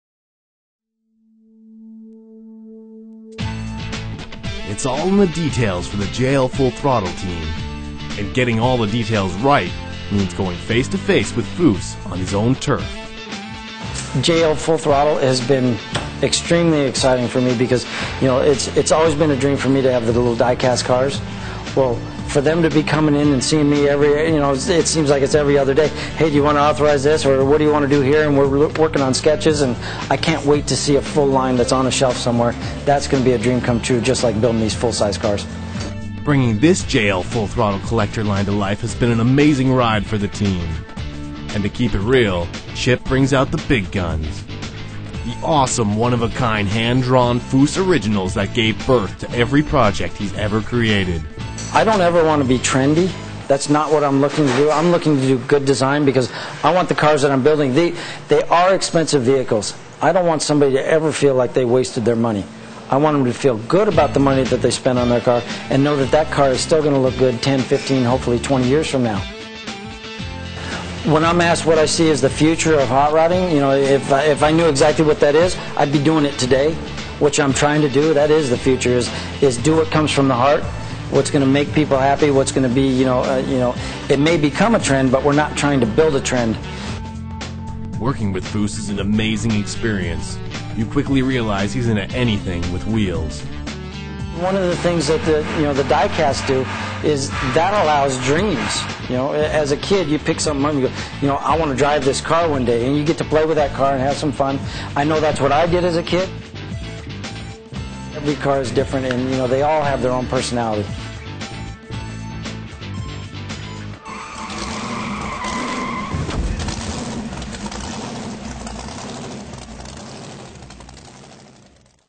Making of JLFT Foose Design Collector Diecast
Details make the difference! Join the JL Full Throttle team as they work on the details of the Foose Design line of diecast with the man himself in his busy shop. Hear his take on the future of hot rodding and more!